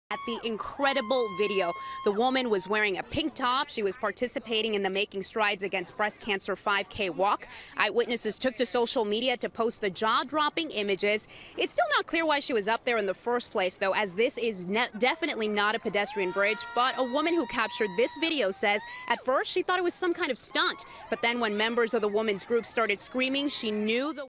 Woman, 55, left clinging for her life 22 feet in the air the moment railroad bridge
A woman in Fort Lauderdale, Florida got stuck on a railroad bridge today, and had to be rescued by local firefighters.
The 55-year-old Dania Beach woman was crossing the New River using a railroad bridge, when the bridge unexpectedly started to lift up.
As the bridge started to rise, the woman held on to the sides of the two tracks to remain aloft